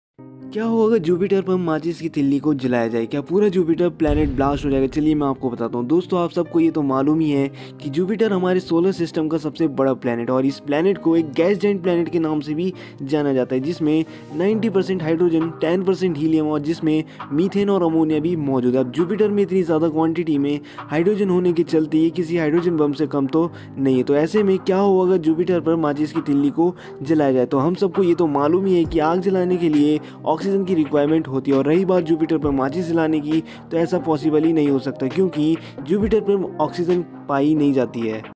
आखिर Jupiter पर माचिस जलाने से क्या होगा 🤯┃
आखिर Jupiter पर माचिस जलाने से क्या होगा 🤯 ┃Interesting facts┃Amazing facts┃

Hello Everyone
Welcome to samratfacts
In Today's video you are going to know the facts about